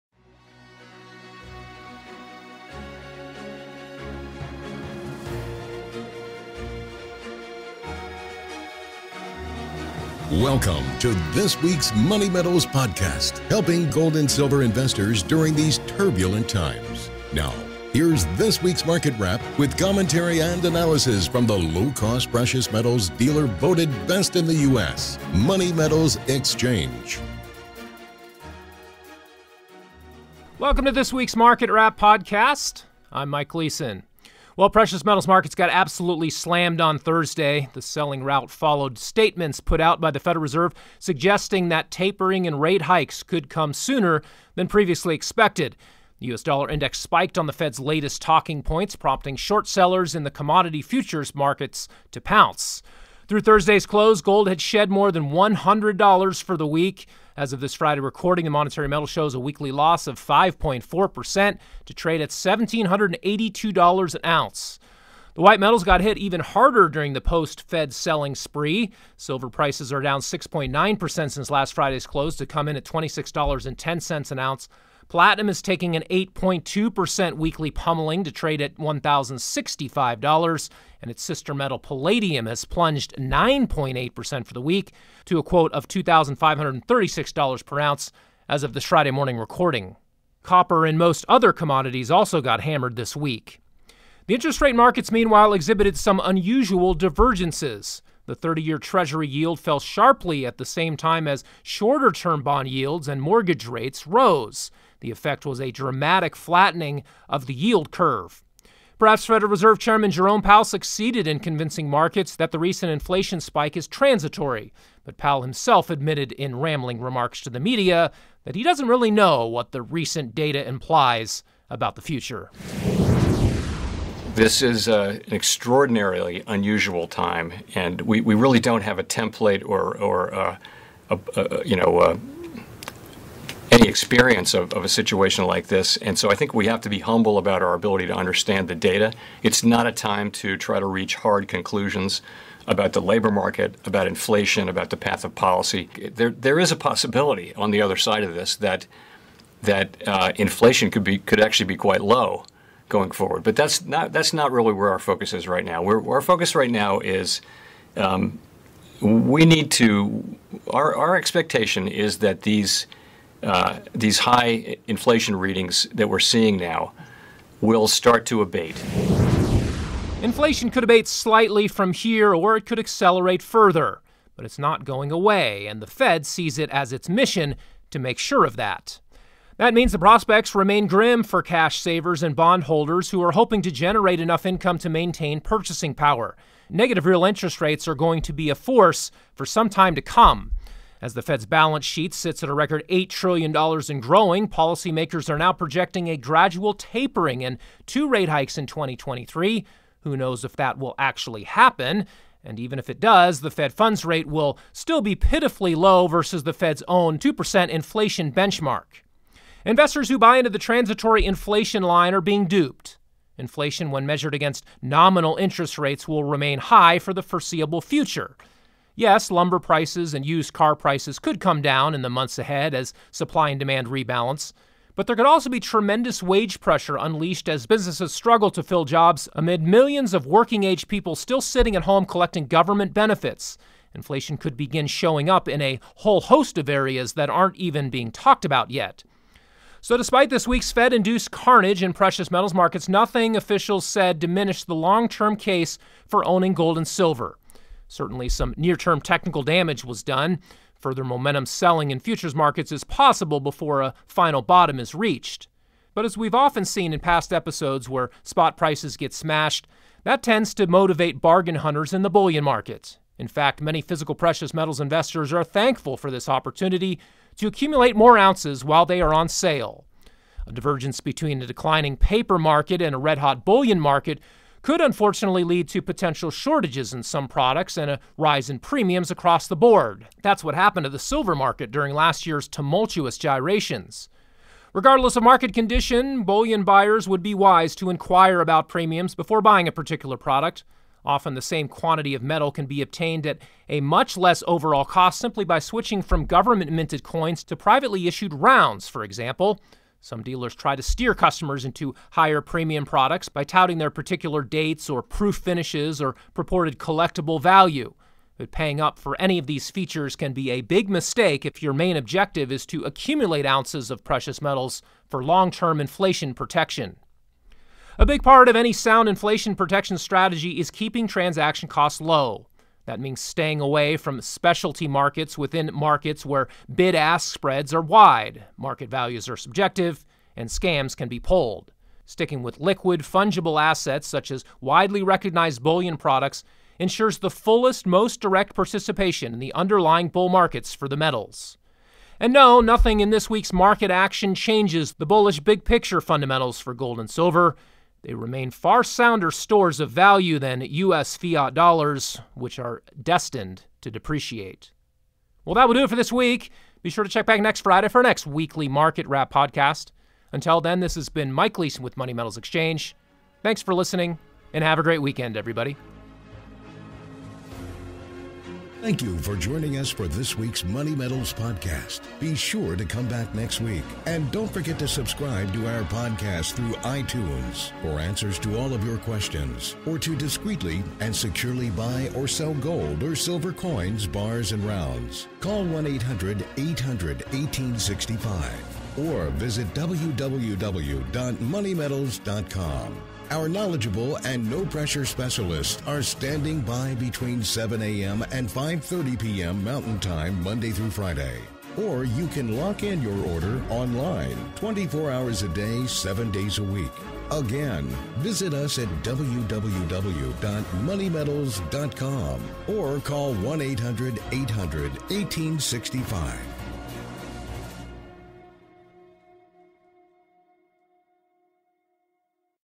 Fed Musings about Distant Interest Rate Hikes Spook Metals Markets
Well, precious metals markets got absolutely slammed on Thursday.  The selling rout followed statements put out by the Federal Reserve suggesting that tapering and rate hikes could come sooner than previously expected. | Do you own precious metals you would rather not sell, but need access to cash?